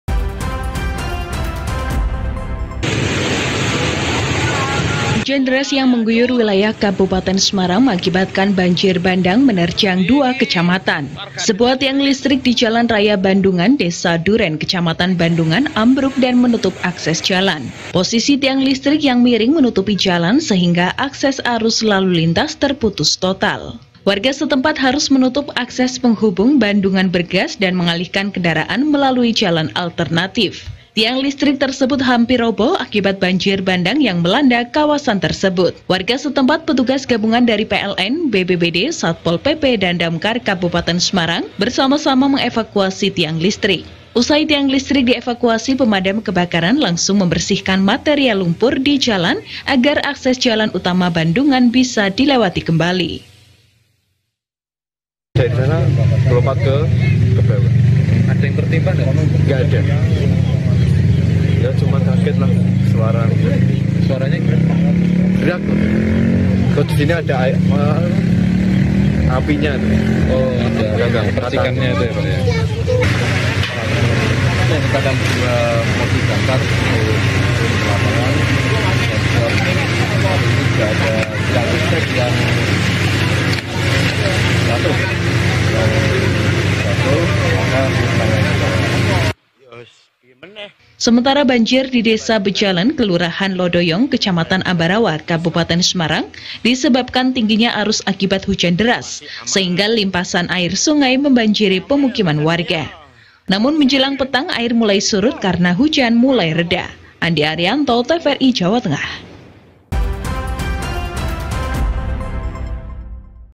Banjir Bandang Terjang Dua Kecamatan di Kabupaten Semarang
JatengTerkini 6 Januari 2024

Dua kecamatan di Kabupaten Semarang diterjang banjir bandang. Banjir bandang mengakibatkan tiang listrik ambruk di Kecamatan Bandungan. Sedangkan di Kecamatan Ambarawa air meluap menggenangi pemukiman warga.